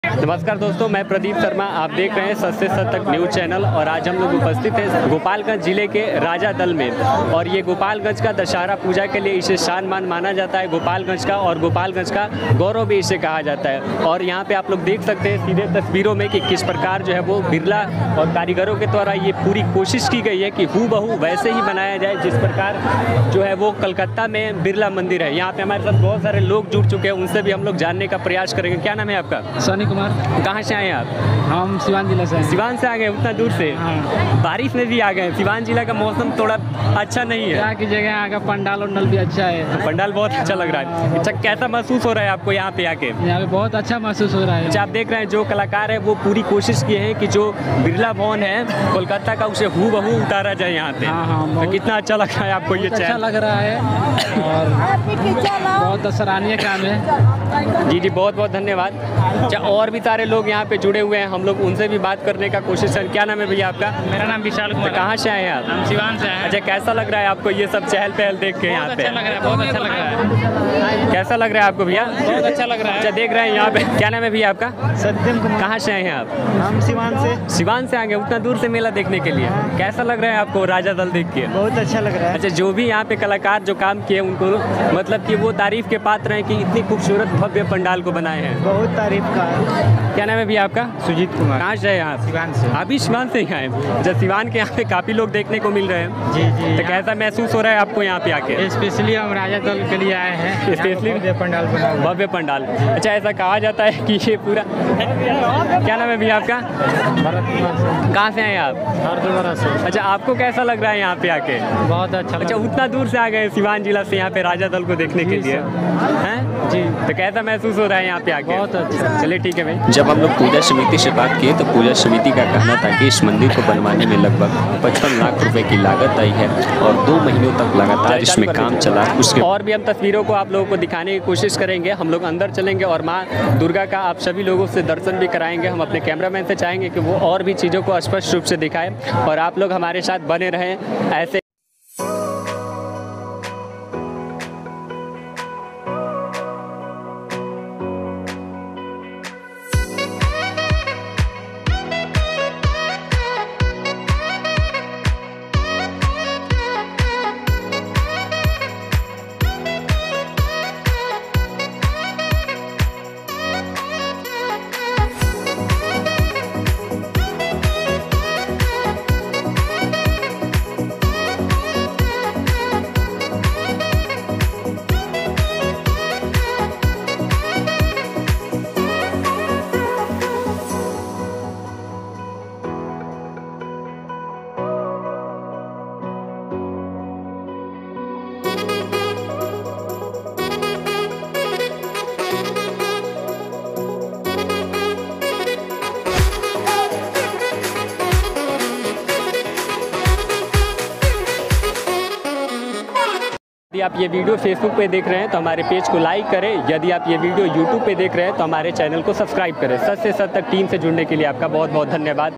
गोपालगंज का राजा दल जिसकी चर्चा पूरे बिहार में हो रही है
Ground Zero :-Pradeep sharma
 Sunil singh
Cameraman-kuldeep kumar

E-mail-suchsesuchtak
Instagram- such se such tak
Facebook- such se such tak